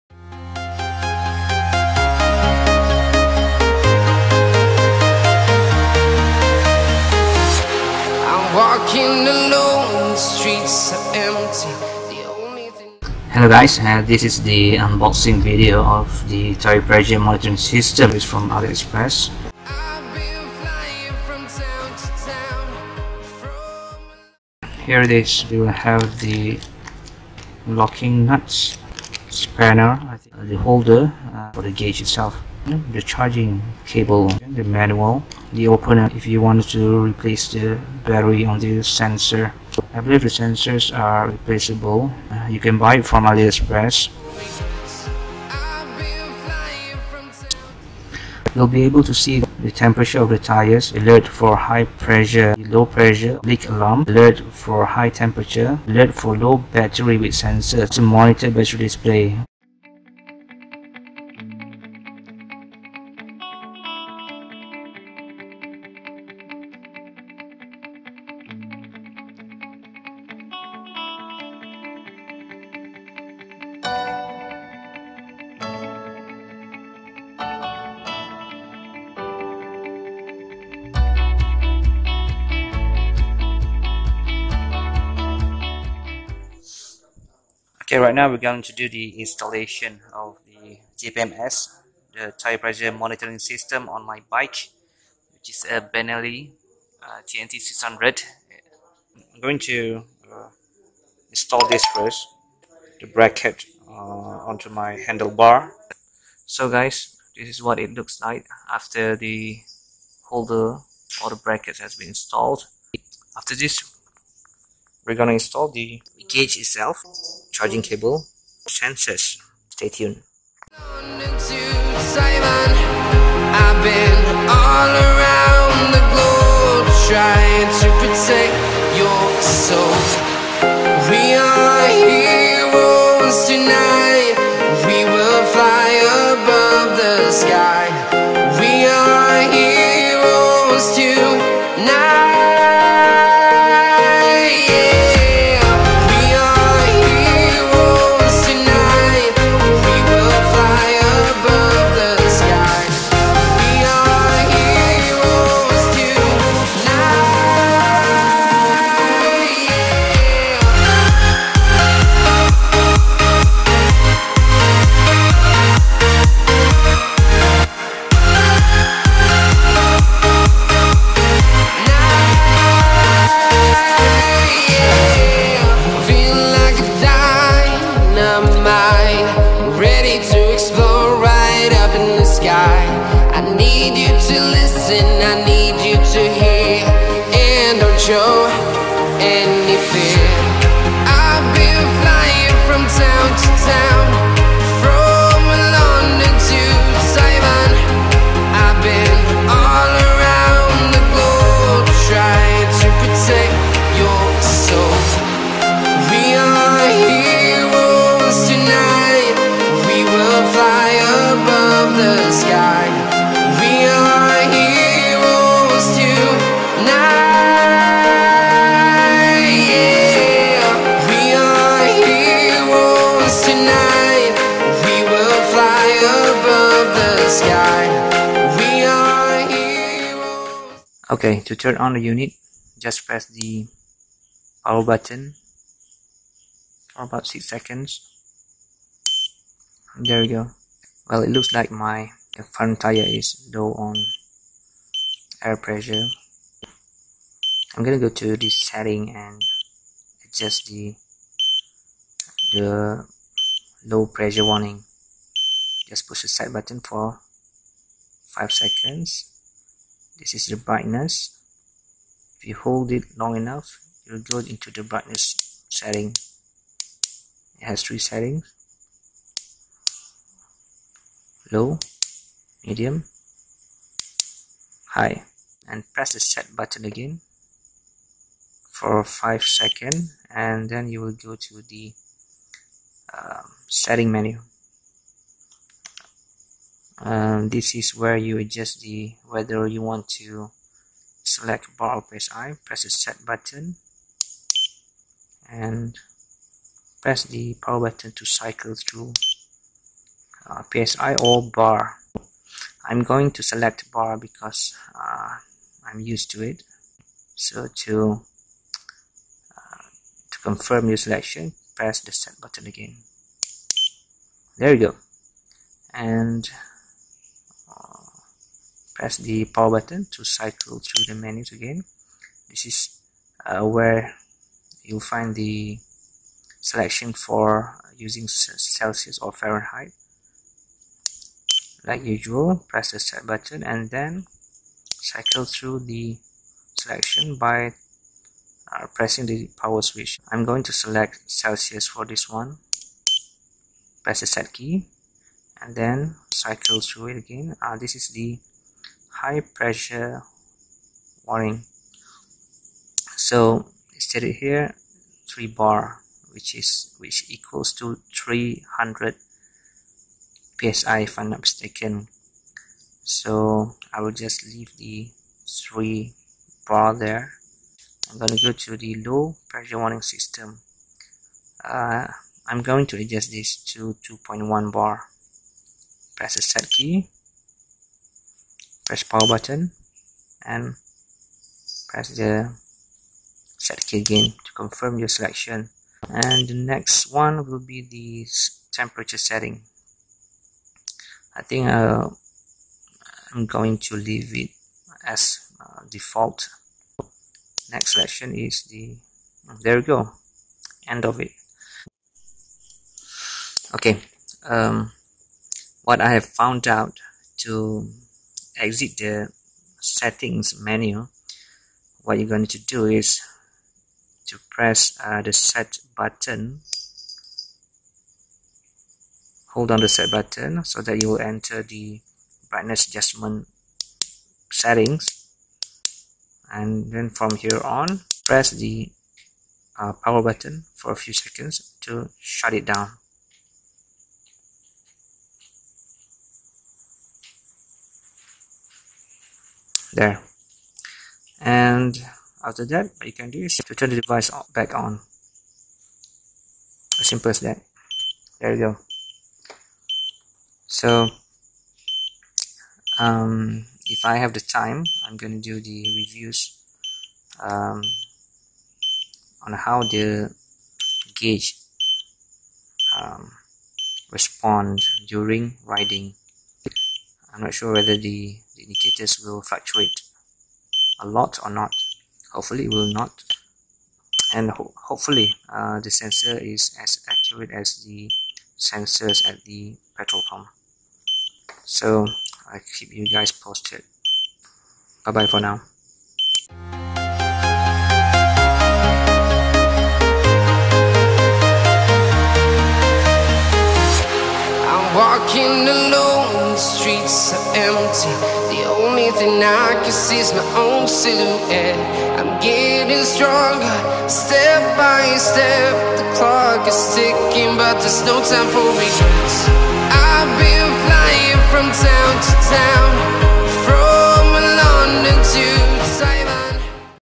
Tyre Pressure Monitoring System TPMS for Motorcycle Installation & Review
TPMS motorcycle bought from AliExpress
Model M3 Wireless Motorcycle TPMS